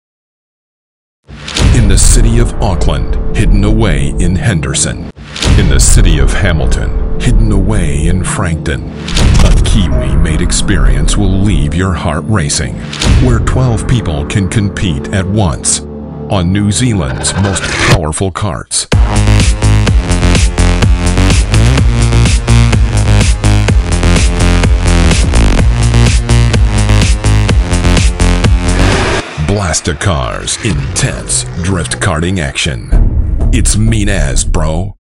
Blastacars Drift Karts New Zealand - Official Trailer (HD)
Blastacars Drift Karts, A Kiwi made Karting experience like no other. Try our unique indoor drift kart tracks in Hamilton or Auckland city today!